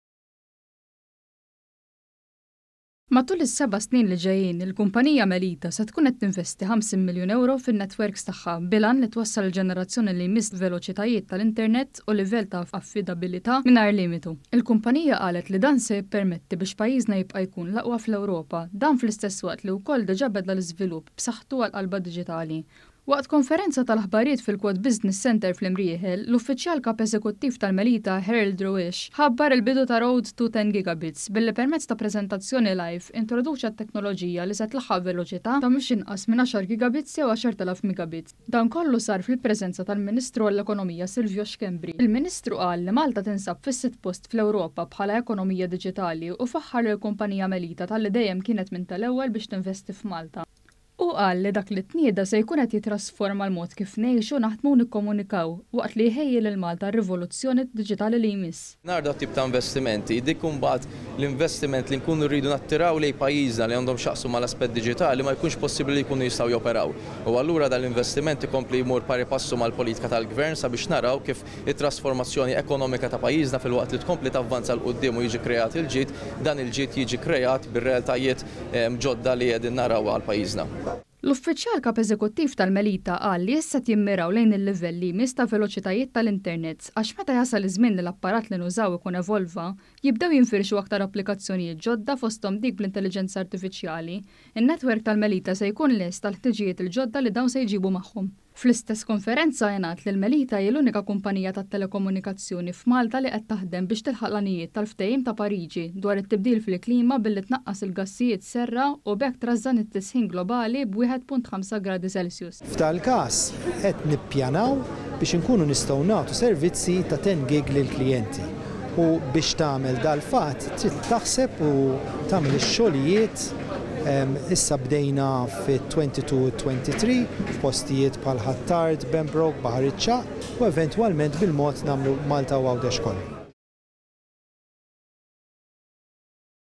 MELITA TINVESTI €50 MILJUN FL-INTERNET LI JMISS
ONE.COM.MT • Dalgħodu tħabbar li l-kumpanija Melita se tinvesti mal-€50 miljun fin-netwerks tagħha matul is-seba’ snin li ġejjin bil-għan li tibda tipprovdi servizzi fil-ġenerazzjoni li jmiss ta’ veloċitajiet tal-Internet f’ekonomija li se tkompli taqleb għal waħda diġitali.

KITBA: NICOLE SCIBERRAS BALBI
FILMATI: ALEX BORG

Matul is-seba’ snin li ġejjin, il-kumpanija Meltia se tkun qed tinvesti €50 fin-netwerks tagħha bil-għan li twassal il-ġenerazzjoni li jmiss ta’  veloċitajiet tal-internet u livell ta’ affidabbiltà mingħajr limitu.

Il-kumpanija qalet li dan se jippermetti biex pajjiżna jibqa’ jkun l-aqwa fl-Ewropa. Dan fl-istess waqt li wkoll diġà beda l-iżvilupp b’saħħtu għall-qalba diġitali.

Waqt konferenza tal-aħbarijiet fil-Quad Business Centre fl-Imrieħel, l-Uffiċjal Kap Eżekuttiv tal-Melita, Harald Roesch, ħabbar il-bidu ta’ ‘Road to 10 Gigabits’ billi, permezz ta’ preżentazzjoni live, introduċa t-teknoloġija li se tlaħħaq veloċità ta’ mhux inqas minn 10 Gigabits – jew 10,000 Megabits. Dan kollu sar fil-preżenza tal-Ministru għall-Ekonomija, Silvio Schembri.

Il-Ministru qal li Malta tinsab fis-sitt post fl-Ewropa bħala ekonomija diġitali u faħħar lill-kumpanija Melita talli dejjem kienet minn tal-ewwel biex tinvesti f’Malta. Qal li dak li tnieda se jkun qed jittrasforma l-mod kif ngħixu, naħdmu u nikkomunikaw, waqt li jħejji lil Malta għar-rivoluzzjoni diġitali li jmiss.

SILVIO SCHEMBRI
MINISTRU
“Mingħajr dawn it-tip ta’ investimenti, dik imbagħad l-investiment li nkunu rridu nattiraw lejn pajjiżna li għandhom x’jaqsmu mal-aspett diġitali, ma jkunx possibbli li jkunu jistgħu joperaw. Allura dan l-investiment ikompli jmur pari passu mal-politika tal-Gvern ħalli naraw kif issir it-trasformazzjoni ekonomika ta’ pajjiżna, filwaqt li tkompli tavvanza ’l quddiem u jinħoloq il-ġid. Dan il-ġid jista’ jinħoloq bir-realtajiet il-ġodda li qegħdin naraw għal pajjiżna.”

L-Uffiċjal Kap Eżekuttiv tal-Melita qal li issa qed jimmiraw lejn il-livell li jmiss ta’ veloċitajiet tal-Internet, għax meta jasal iż-żmien li l-apparat li nużaw ikun evolva jibdew jinfixru aktar applikazzjonijiet ġodda, fosthom dik bl-intelliġenza artifiċjali, in-netwerk tal-Melita se jkun lest għall-ħtiġijiet il-ġodda li dawn se jġibu magħhom.

Fl-istess konferenza ngħad li l-Melita hi l-unika kumpanija tat-telekomunikazzjoni f’Malta li qed taħdem biex tilħaq l-għanijiet tal-Ftehim ta’ Pariġi dwar it-tibdil fil-klima billi tnaqqas il-gassijiet serra u b’hekk trażżan it-tisħin globali b’1.5 gradi Celsius.

SIMON MONTANARO
UFFIĊJAL KAP TAT-TEKNOLOĠIJA, MELITA
“Qed nippjanaw biex inkunu nistgħu nagħtu servizzi 10 gig lill-klijenti u biex tagħmel dan il-pass trid taħseb u tagħmel ix-xogħlijiet. Issa bdejna bi 22 u 23 f’postijiet bħal Ħ’Attard, Pembroke u Baħar iċ-Ċagħaq u eventwalment bil-mod nagħmlu Malta u Għawdex kollu.”